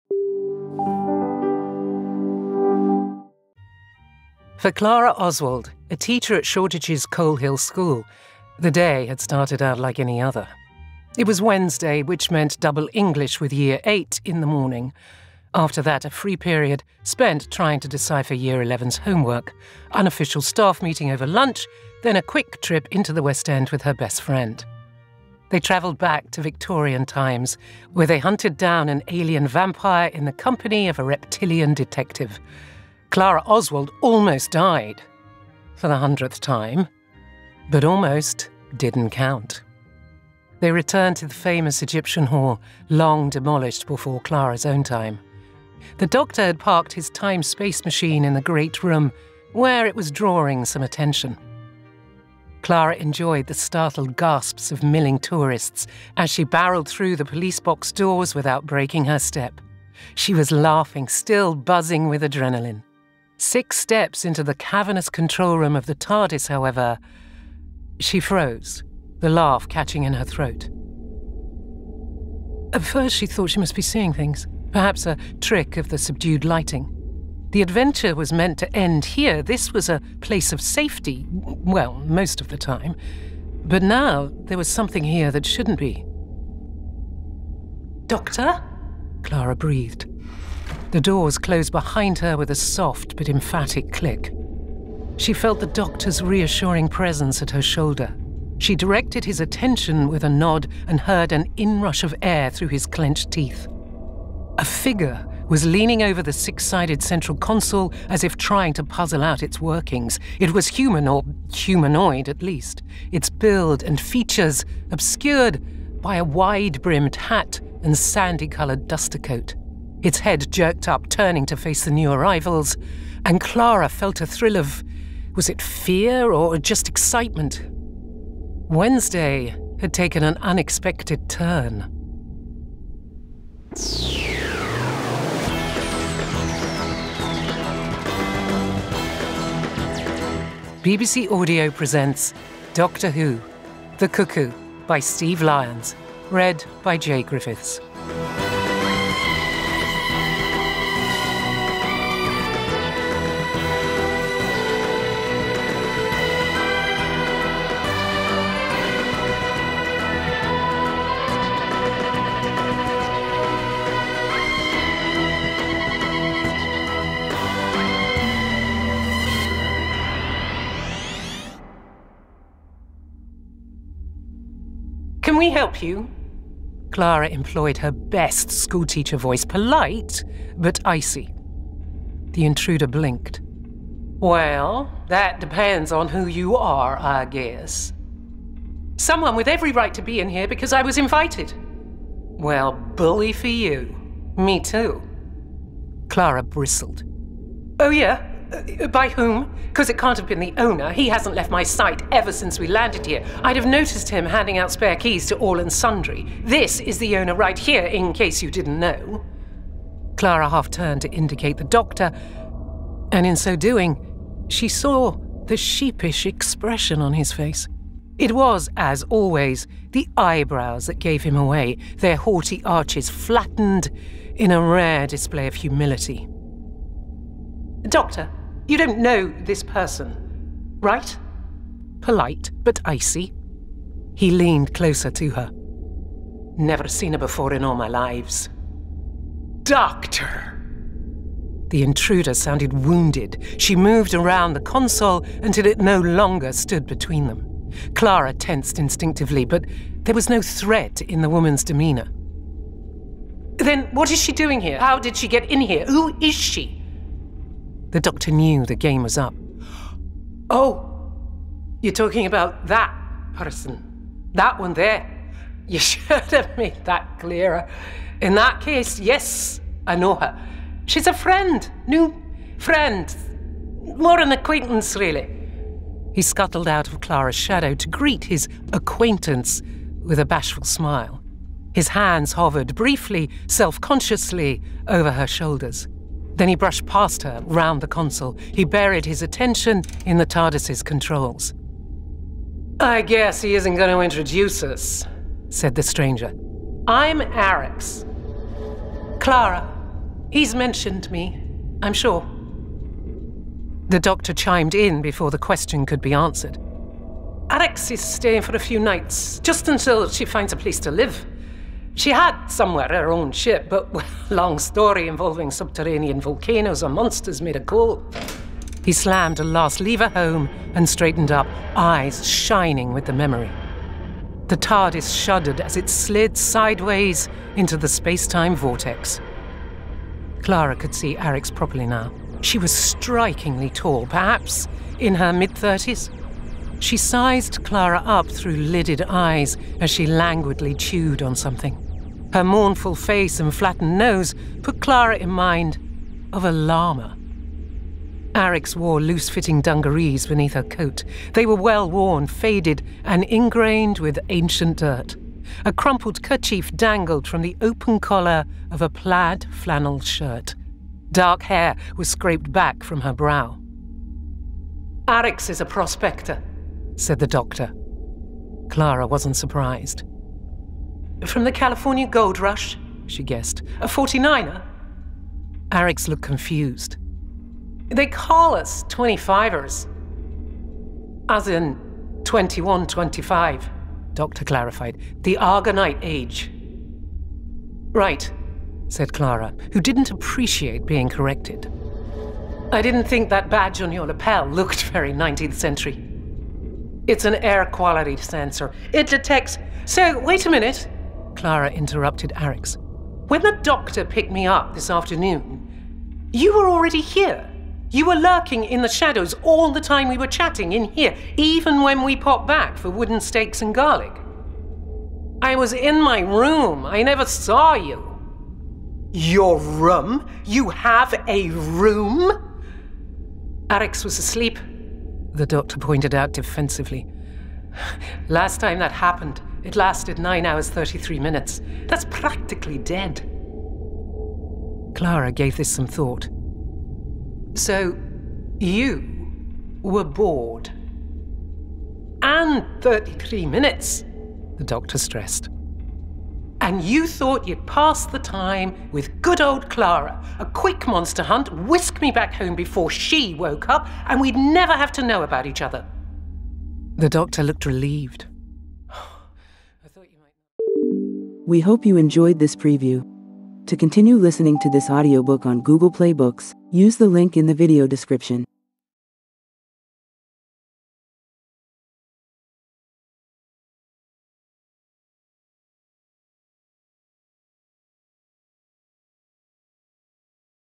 Doctor Who: The Cuckoo: 12th Doctor Audio… by Steve Lyons · Audiobook preview
Doctor Who: The Cuckoo: 12th Doctor Audio Original
Authored by Steve Lyons
Narrated by Jaye Griffiths

0:00 Intro
0:03
7:40 Outro

BOOK DESCRIPTION

aye Griffiths reads this original adventure for the Twelfth Doctor and Clara, as played on TV by Peter Capaldi and Jenna Coleman.

For Clara Oswald, life has fallen into a routine. By day she teaches the pupils at Coal Hill School, and by night – and at weekends – she travels in Time and Space with the Doctor.

Clara's having the time of her life, enjoying all the thrill and laughter of being with the Doctor – until, one day, there's somebody else.

To Clara's immense shock, the Doctor has acquired a new companion – and Arix is just as at home in the TARDIS as she has been. Suddenly, Clara's world has tilted on its axis.
As the two companions size each other up, the Doctor is caught between them. And when they all wind up in an alien swamp, it's time for a reckoning of grievances, grudges – and bravery.
Jaye Griffiths, who played Jac in the Doctor Who TV series, reads Steve Lyons's compelling and moving character piece.

Reading produced by Neil Gardner/Ladbroke Audio.
Sound design by David Roocroft.
Executive producer for BBC Audio: Michael Stevens.

AUDIOBOOK DETAILS

Language: English
Publisher: BBC Digital Audio
Published on: December 7, 2023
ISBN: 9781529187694
Duration: 1 hr, 16 min
Genres: Fiction / Science Fiction / Alien Contact, Fiction / Science Fiction / General, Fiction / Science Fiction / Time Travel